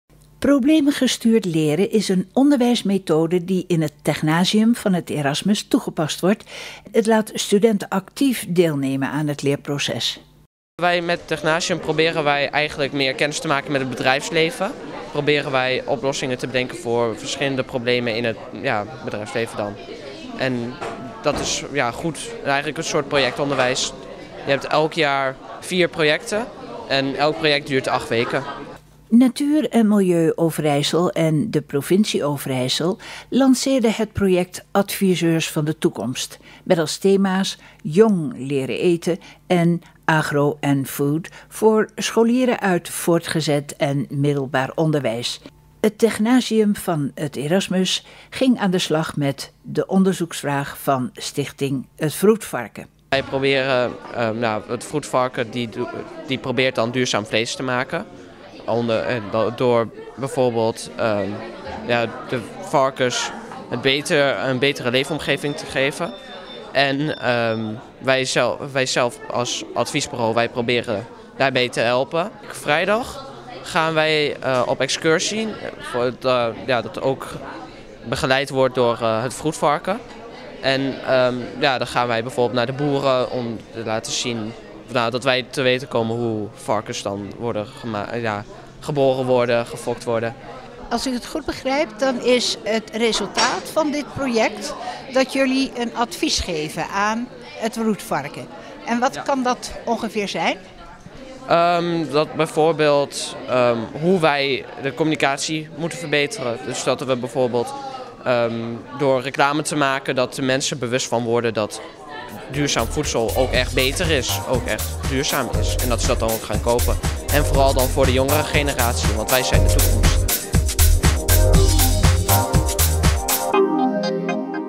Technasium Erasmus adviseert Het Wroetvarken
Technasium van Het Erasmus ging aan de slag met de onderzoeksvraag van Stichting Het Wroetvarken.